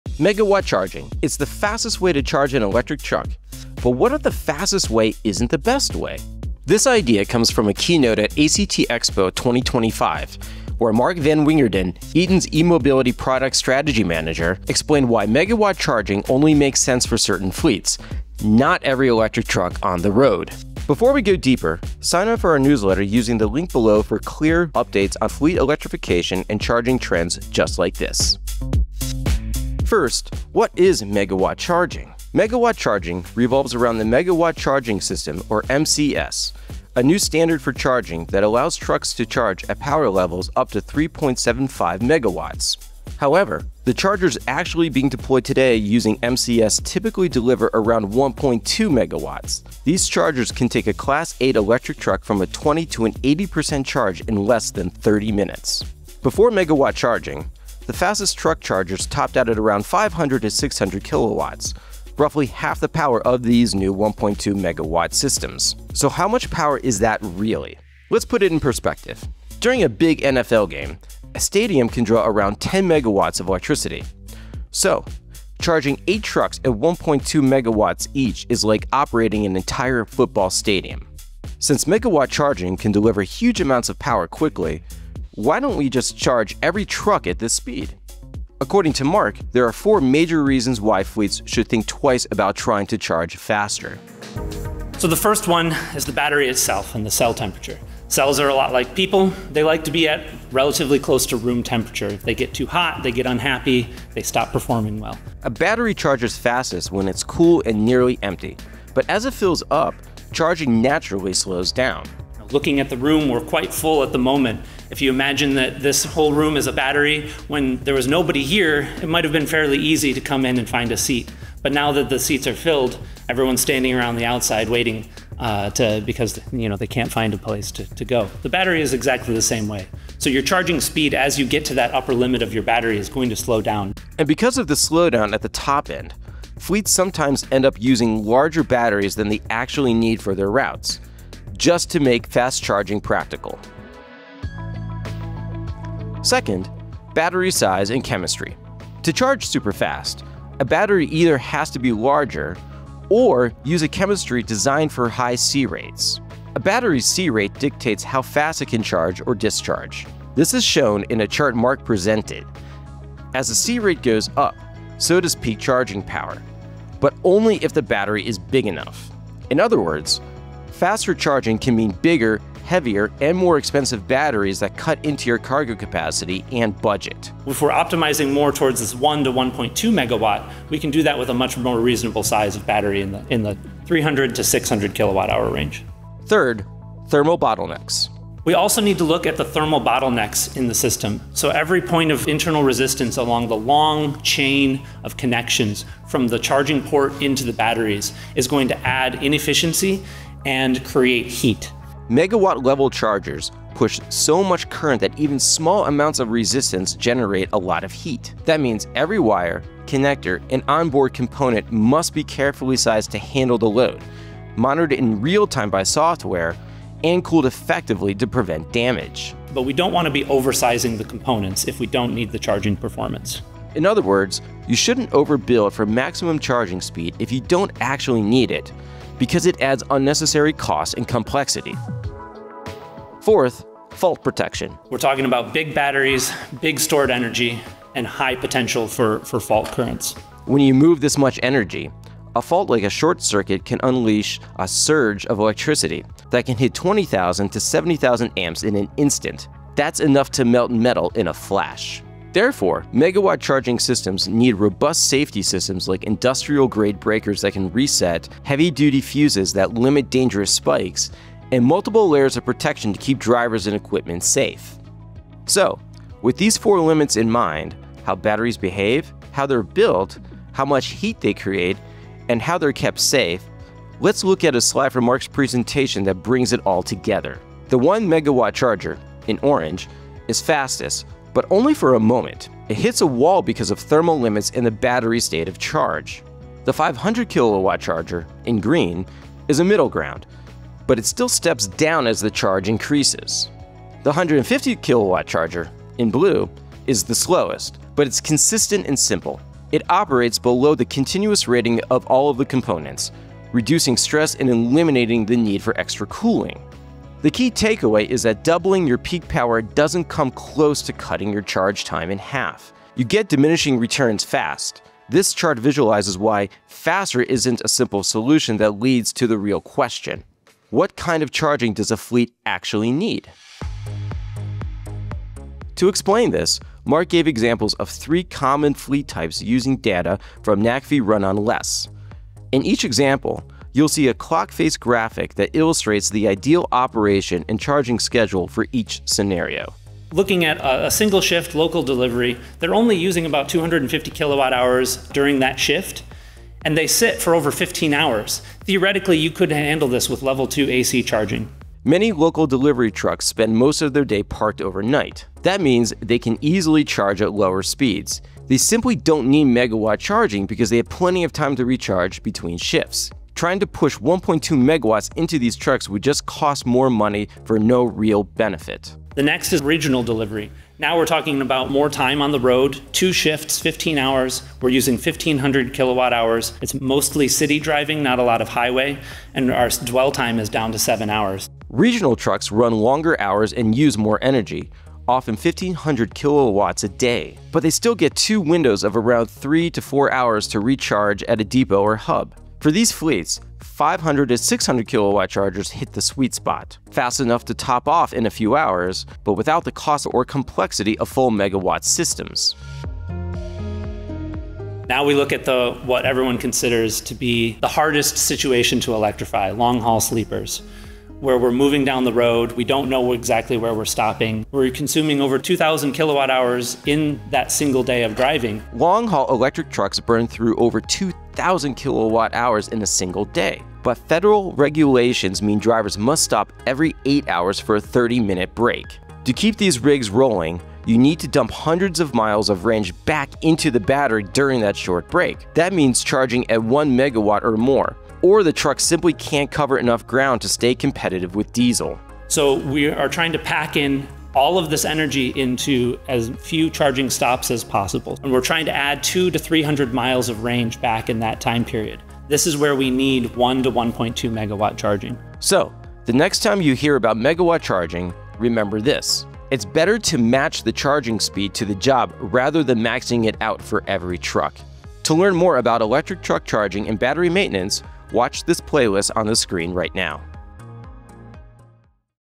Faster Isn’t Always Better: Megawatt Truck Charging Explained
for more electric truck charging and fleet electrification insights like this.

Megawatt charging for electric trucks is finally here, a long-awaited milestone for those in the fleet management business. However, at ACT Expo 2025, Eaton’s eMobility Product Strategy Manager, Mark Van Wingerden, claimed in a keynote that megawatt charging is not the ultimate charging solution fleet managers have been waiting for.

But first, what is megawatt charging? Megawatt charging is based around a new charging standard, called the Megawatt Charging System (MCS). This standard allows for charging up to 3.75 megawatts. However, current chargers beginning to come onto the market today offer up to 1.2 megawatts. This is double the power of the maximum charging power that was previously available to fleets, which was 500-600 kW.

While double the power sounds amazing, Mark’s keynote at ACT Expo made the argument that charging speeds need to be matched to a vehicle’s job instead of always being maxed out. His argument revolves around 4 bottlenecks associated with electric truck charging. These bottlenecks can be overcome, but they add cost and complexity to fleet electrification and should only be tackled in certain scenarios.

So, with these 4 bottlenecks in mind, what kinds of fleets really need megawatt charging? Watch the video to find out.

In This Video:
00:00 – Megawatt Charging: Is Faster Better?
00:30 – What Is Megawatt Charging?
01:35 – Charging Bottlenecks: Temperature and SoC
02:25 – Charging Bottlenecks: Size and Chemistry
03:06 – Charging Bottlenecks: Heat
03:52 – Charging Bottlenecks: Fault Protection
04:25 – Why Faster Isn’t Always Better
05:20 – What Fleets Need Megawatt Charging?
05:40 -  Local Delivery Fleets
06:10 – Regional Delivery Fleets
06:43 – Long-Haul Sleeper Fleets
07:35 – Match Charging Speed to the Job